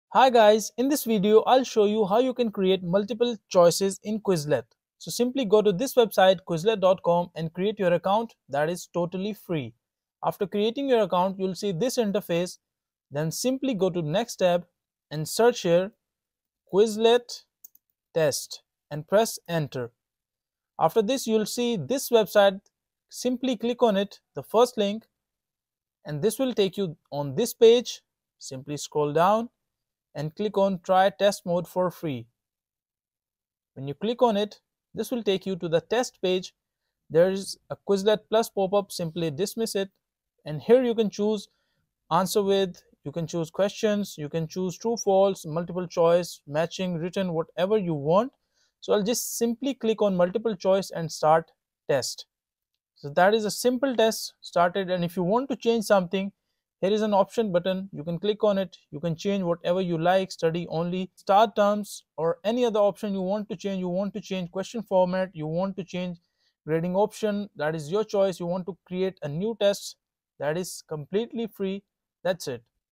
How To Create MULTIPLE CHOICE On Quizlet 2025! (FULL GUIDE)
In this video, I am going to show you how to create multiple choice on Quizlet quickly and easily. Whether you are designing a study set, quiz, or practice test, this step-by-step guide will help you add multiple-choice questions to your Quizlet sets for better learning and engagement.

0:00 INTRO
0:05 TUTORIAL